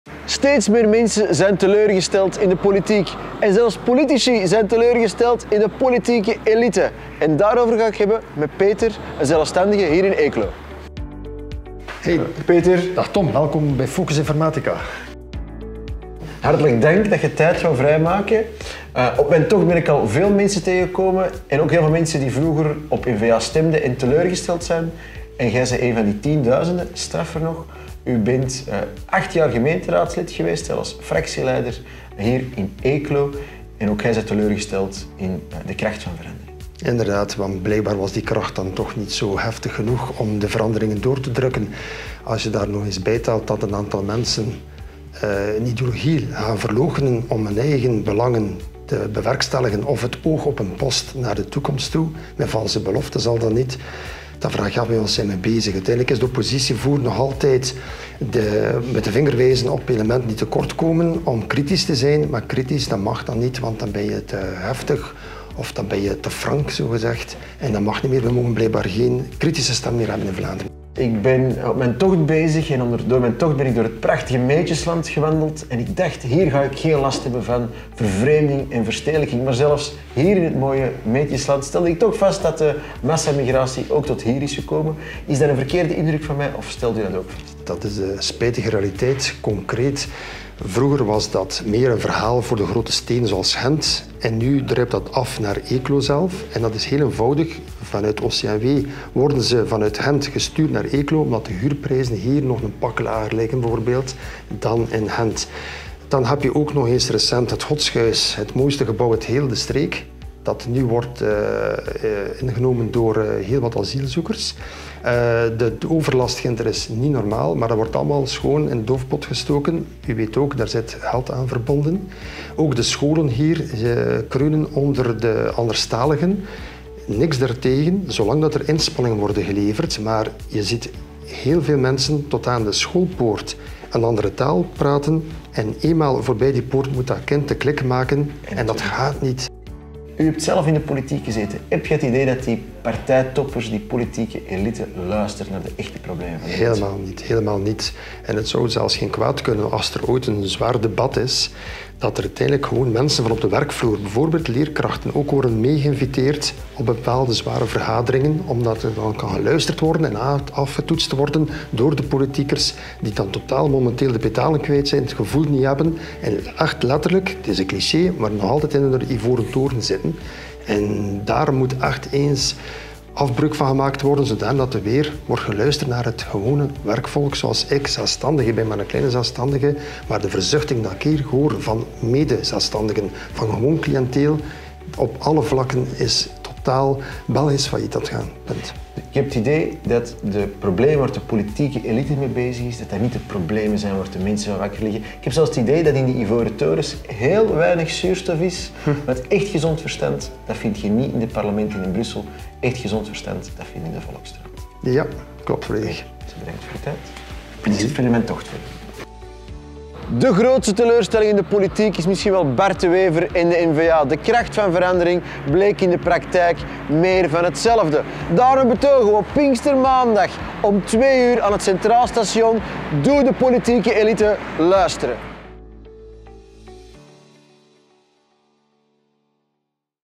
Echte verandering komt er enkel met het Vlaams Belang!
Hoewel de N-VA reeds jarenlang aan de macht is in Vlaanderen, blijft échte verandering uit. Onze voorzitter Tom Van Grieken ging in gesprek met een voormalig fractieleider van de N-VA in Eeklo over zijn visie op de N-VA en de problemen die hij in zijn regio ondervindt. Het is tijd om het systeem echt te veranderen. Dat kan enkel met een sterk Vlaams Belang.